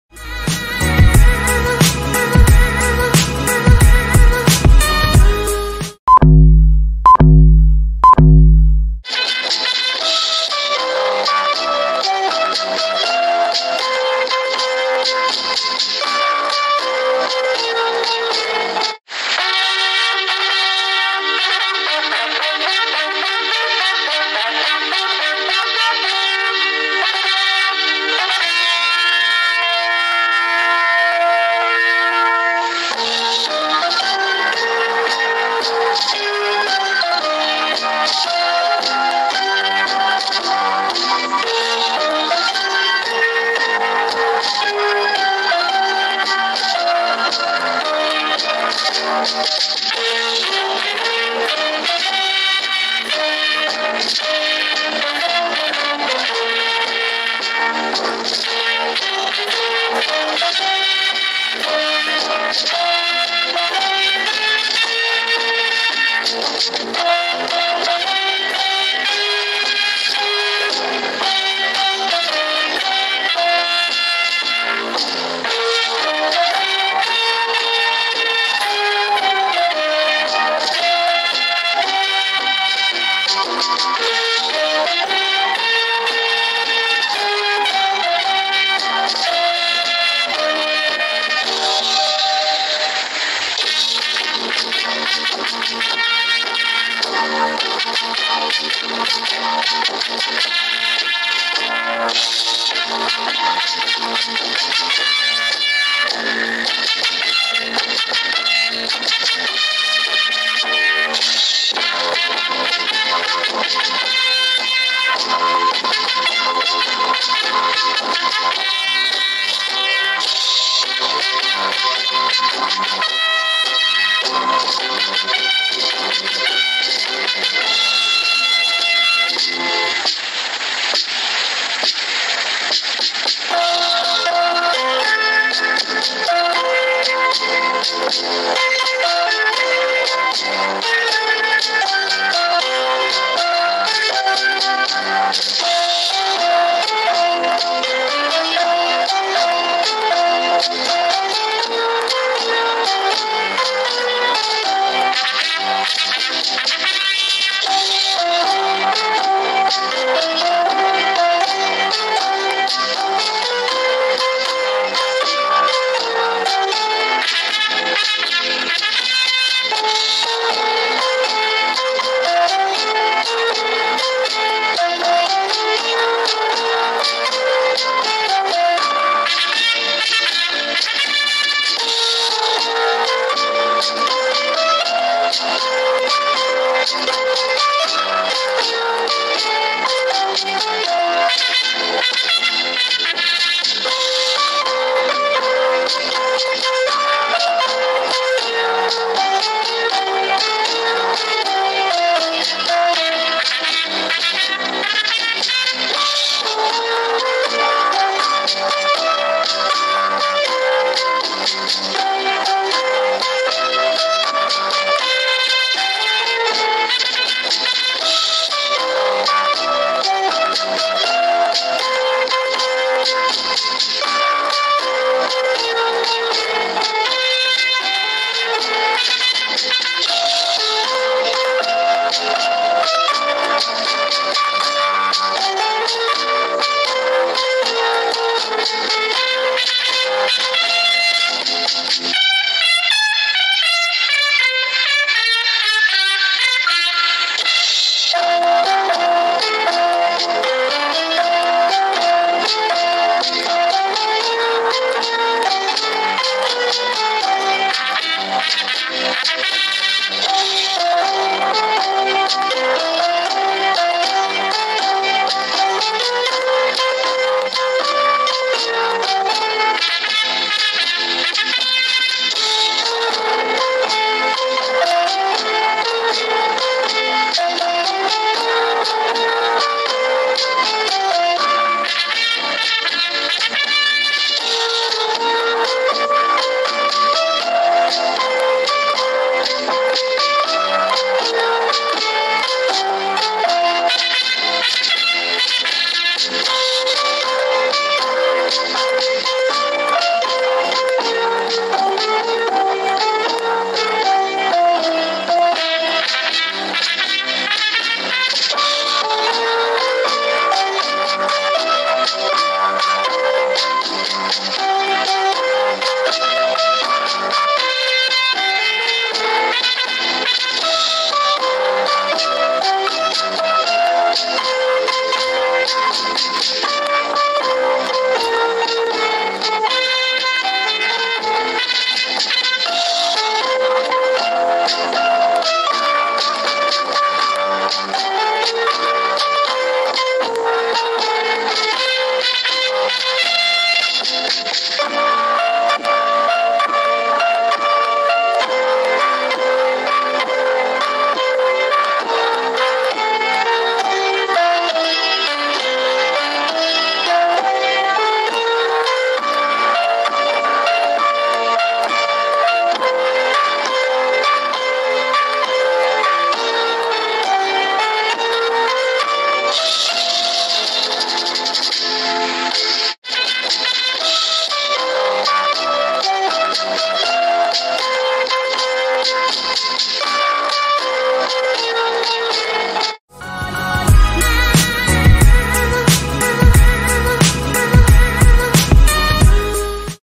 KUDU KEPO! SENAM PAGI INDONESIA SERI D 2025 PALING SUPER MANTUL
Judul: KUDU KEPO! SENAM PAGI INDONESIA SERI D 2025 PALING SUPER MANTUL

Instruktur: Sulung Pambrasto

Salam olahraga! 💪
Senam Pagi Indonesia Seri D jelas paling mantul abis! Mari kita biasakan berolahraga Senam Pagi Indonesia Seri D 2025 setiap hari mulai sekarang, supaya badan kita bertambah sehat bugar. Semoga bermanfaat...! 😀🙏

setelah selesai menonton video ini sekarang.

Salam SKJ, 💪
SULUNG PAMBRASTO